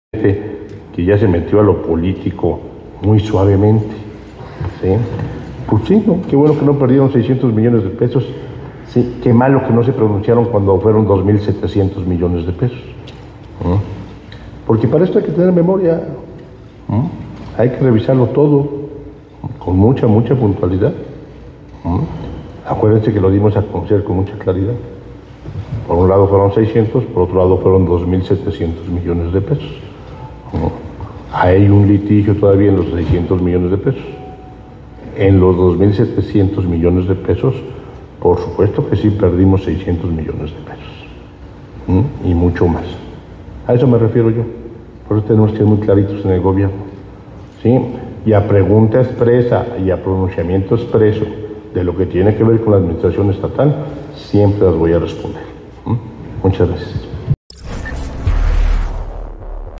Hay que tener memoria: Sergio Salomón a Lalo Rivera ante críticas por la inversión del banco Accendo
🧿"Hay que tener memoria", así respondió el gobernador de Puebla Sergio Salomón Céspedes al edil Eduardo Rivera ante críticas por la inversión del banco Accendo

🧿"Que bueno que no perdieron 600 mdp, que malo que no se pronunció por los 2,700 mdp que se perdieron", criticó el ejecutivo

📹Alejandro Camacho